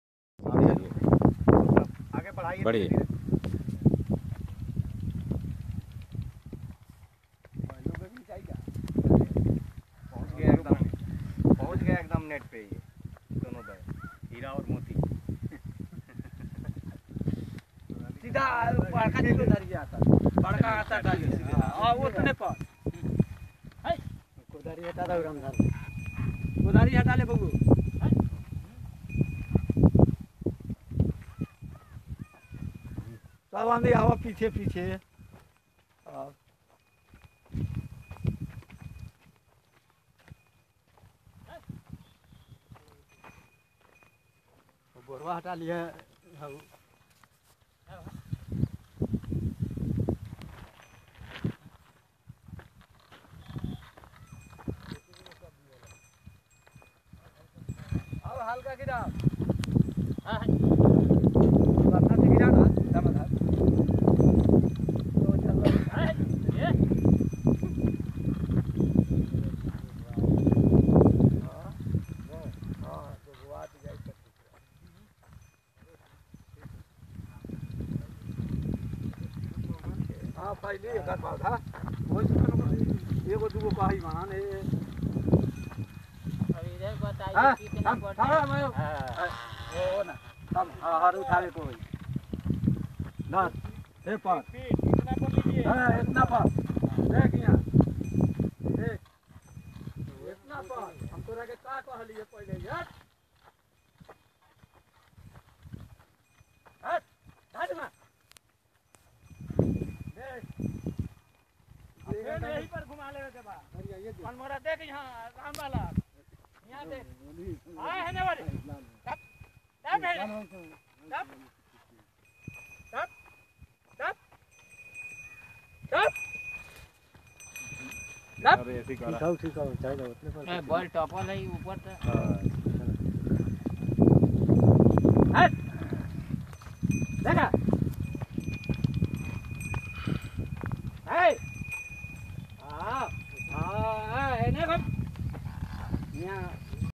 Promoting Pulse (Zayad Moon) Farming in Pakri Dayal, East Champaran, Bihar
Implementing the best practices for Zayad Moong farming through demonstration at Pakri Dayal Block of East Champaran district of Bihar.